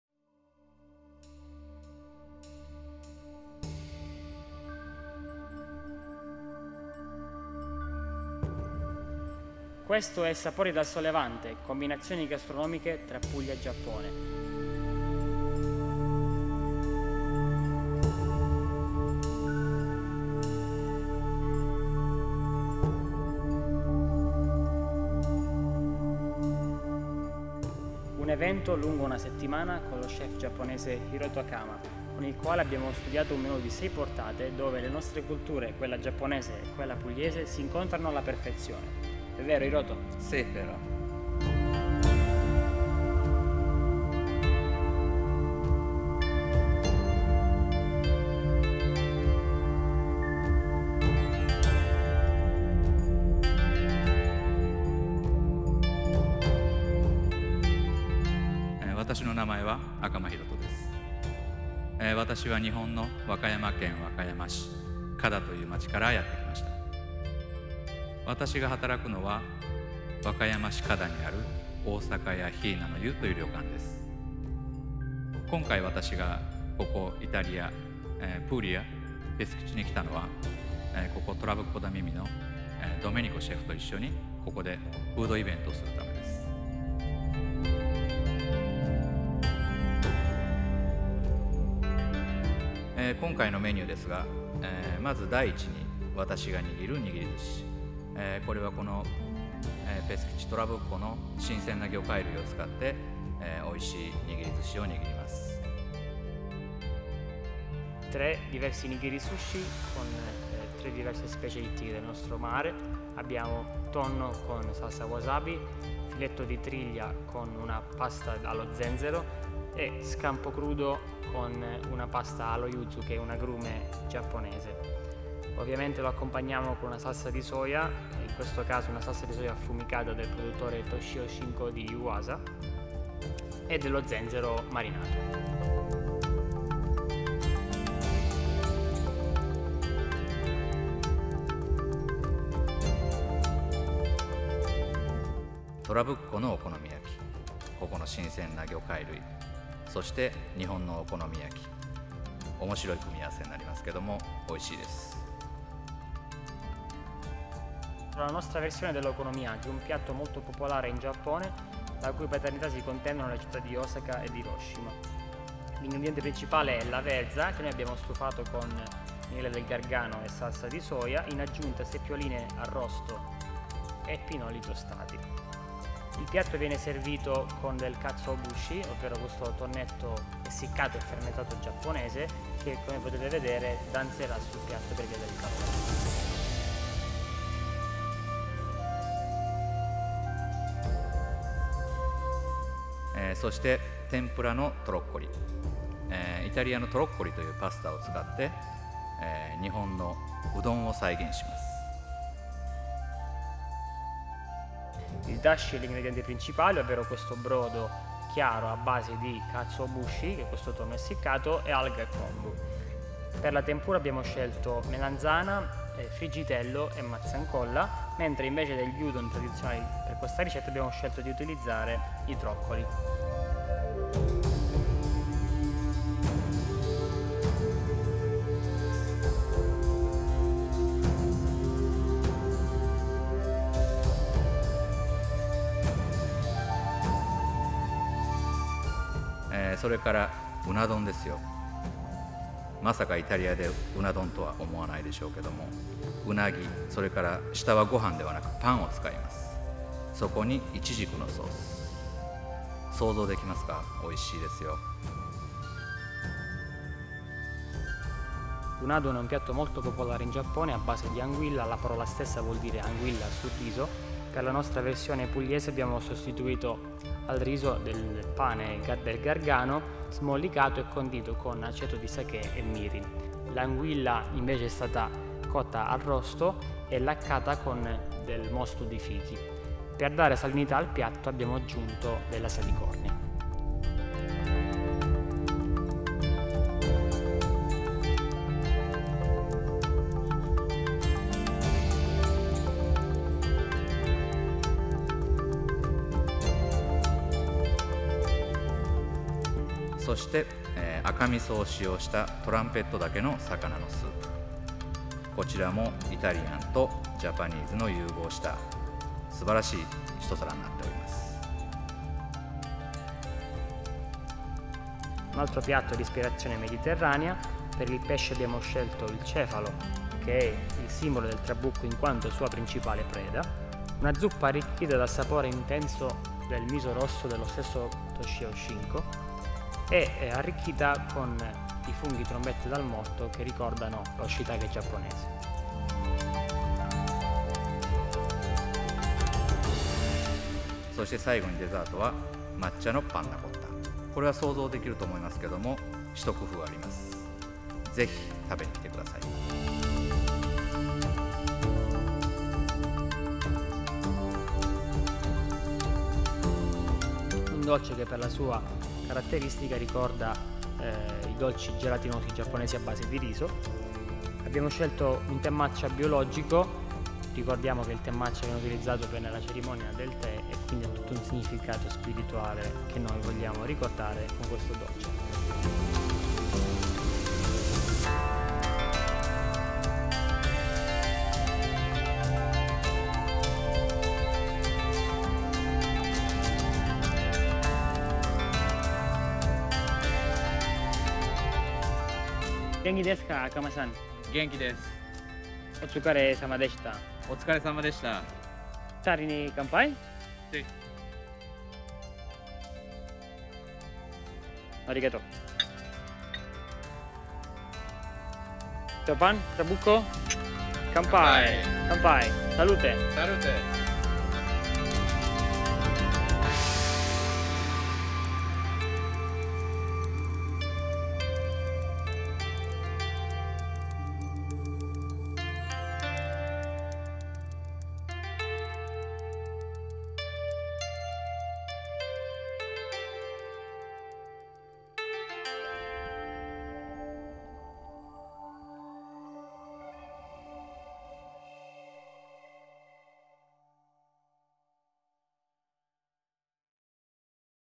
Sapori dal Sol Levante Recolor
~ Per questo evento sono stato incaricato dal Ristorante al Trabucco da Mimì di documentare, con un video e un reportage fotografico completo, il lavoro degli chef Hiroto Akama e Domenico Ottaviano, alle prese con un menu estremamente originale e particolare, nato dall’unione della tradizione gastronomica giapponese con quella pugliese.

Hiroto Akama, oltre a essere un grande professionista e uno chef di fama internazionale, si è mostrato anche una persona estremamente gentile, disponibile e rispettosa, che ha allietato il mio lavoro, permettendomi di realizzarlo nel migliore dei modi, seppur con i ritmi “scanditi” del ristorante, come di consuetudine al Trabucco.

Il risultato è stato un video di 7 minuti, per il quale ho curato anche la realizzazione della musica originale, che ripercorre tutte le portate del menu, accompagnato da una serie di scatti raffiguranti tanto le fasi in cucina quanto quelle in sala.

~ ANNO: 2017

~ CLIENTE:
Ristorante Al Trabucco Da Mimì

~ INCARICO:
Realizzare un video che ripercorresse tutte le portate del menu studiato per l’evento “Sapori dal Sol Levante”

~ COMPETENZE:
Ripresa video
Editing video [Adobe Premiere Pro]
Color grading [DaVinci Resolve Studio]
Fotografia
Editing fotografico [Adobe Lightroom]
Riprese audio
Soundtracking - Composizione
Sound Design
Editing audio [Cubase Pro]